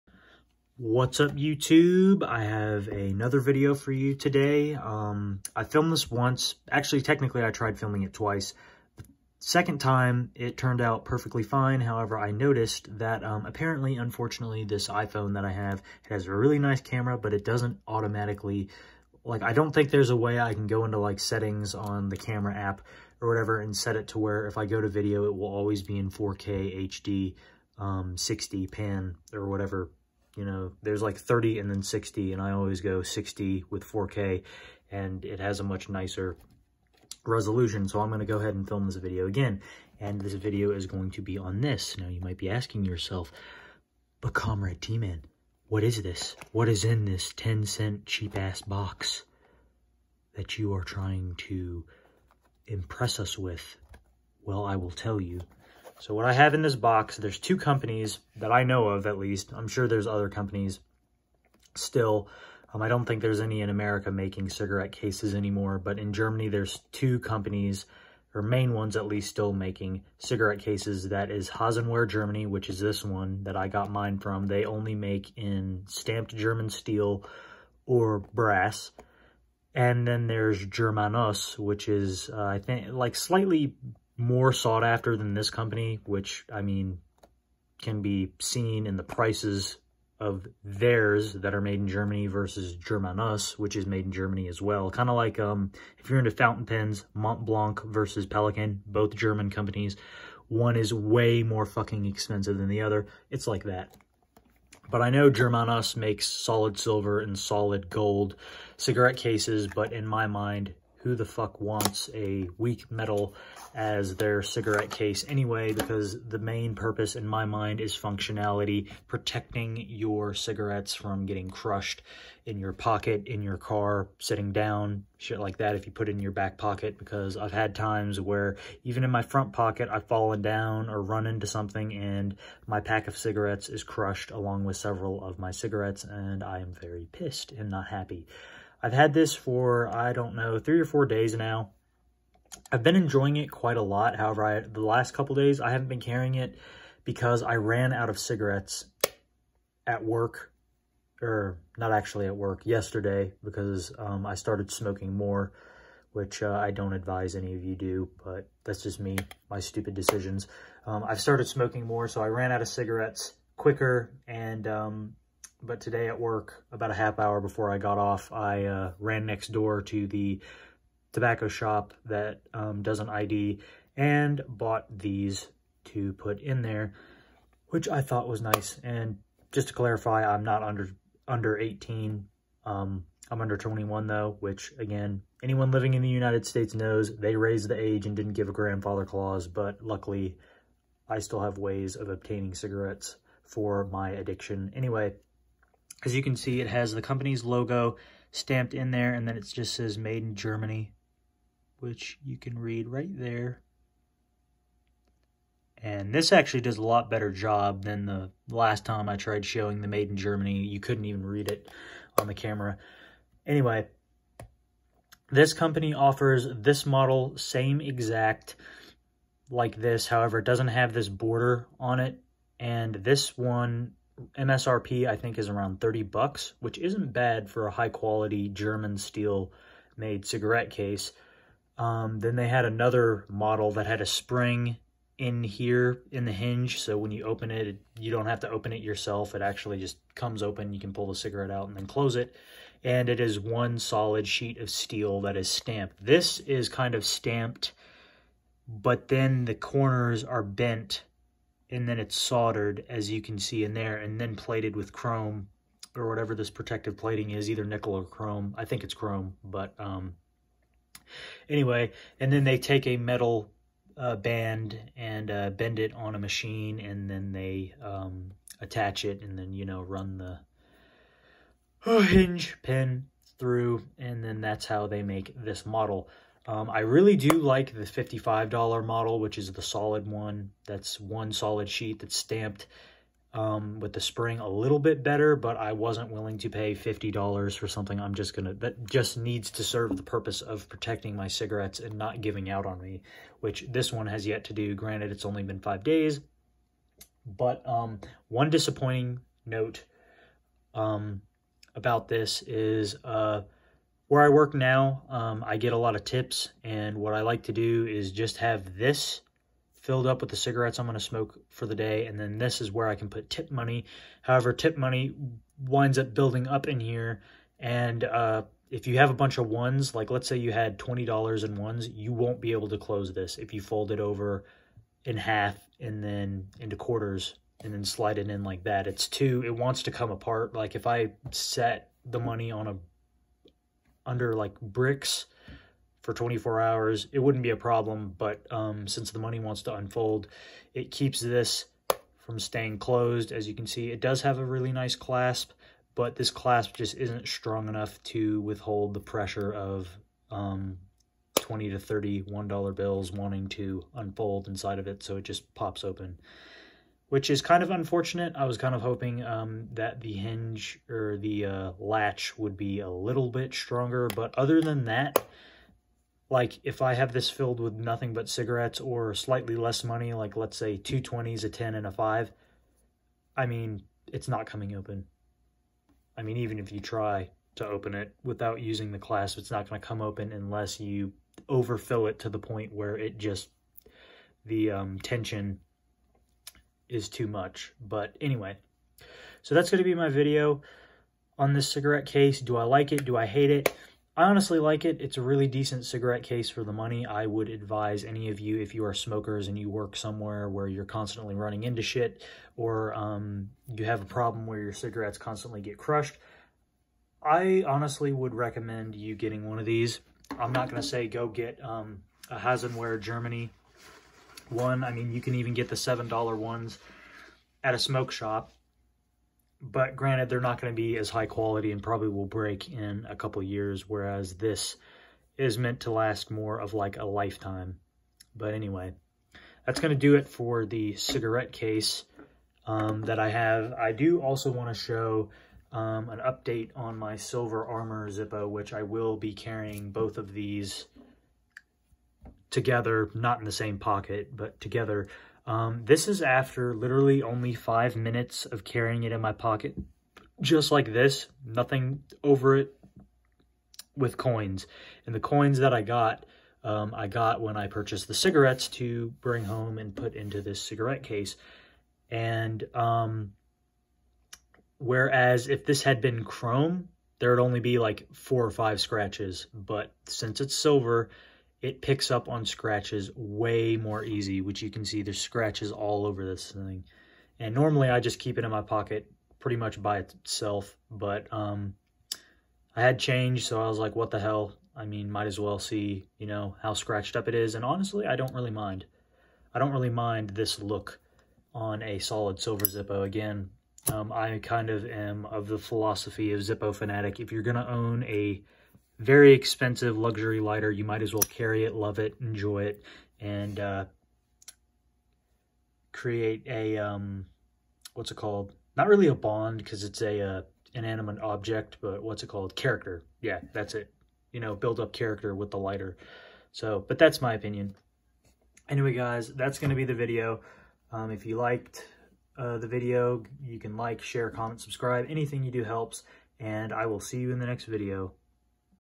German Cigarette Case
Just a quick video on a cig case I got .